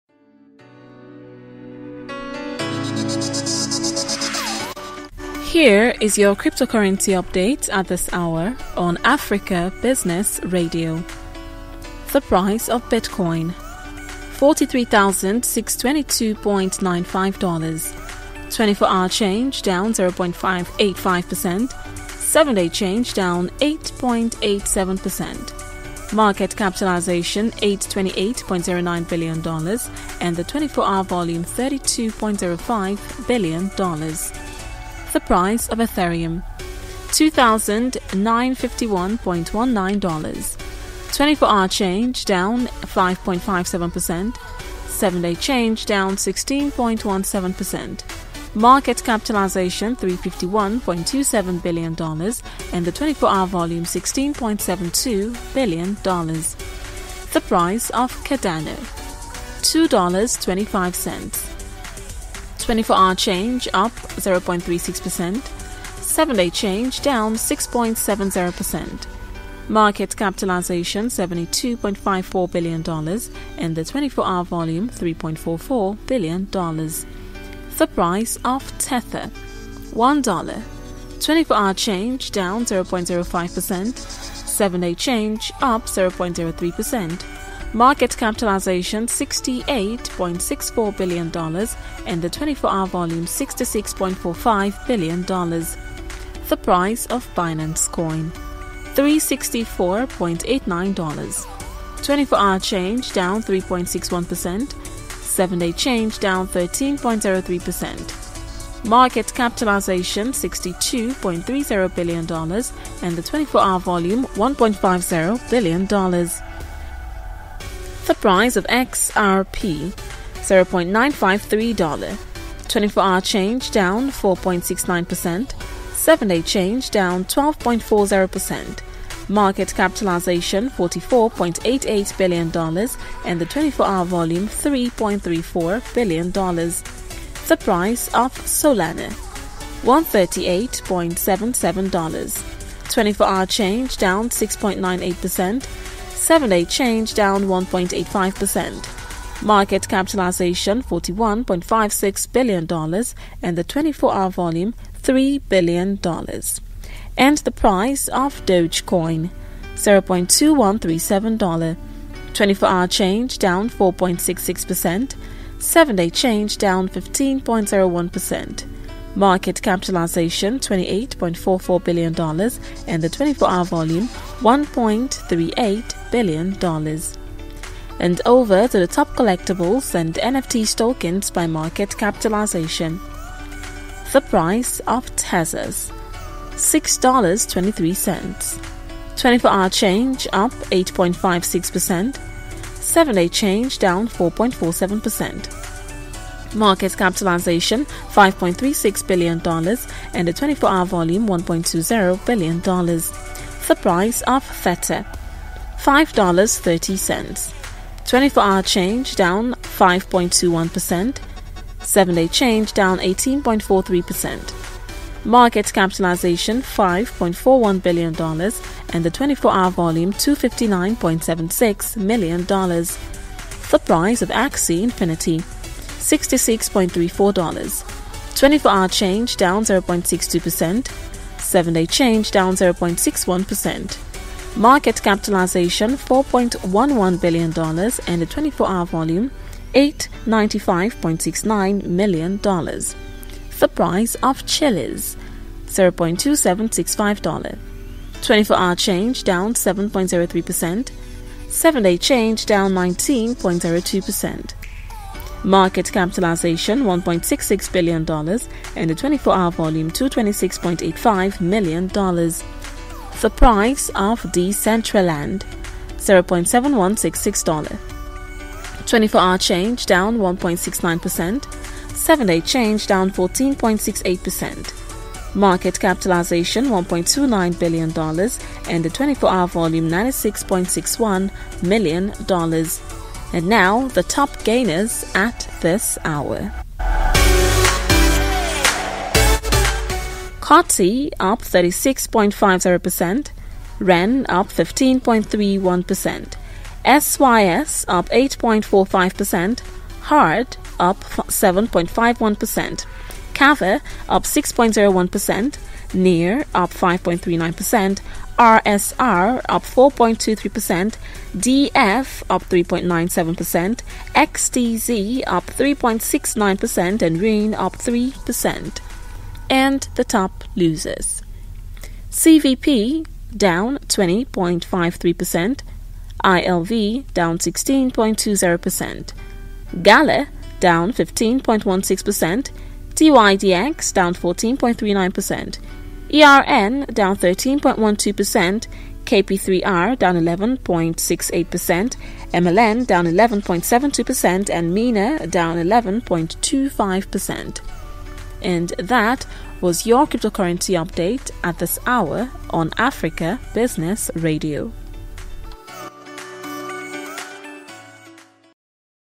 Crypto Currency Update For Mid morning Tuesday 24th 2021
Top Gainers

CORTI up 36.50%, REBN up 15.31%, XYS up 8.41%, HARD up 7.51%, NEAR up 5.39% DF up 3.97%, XTZ up 3.69%,  RENE up 3 %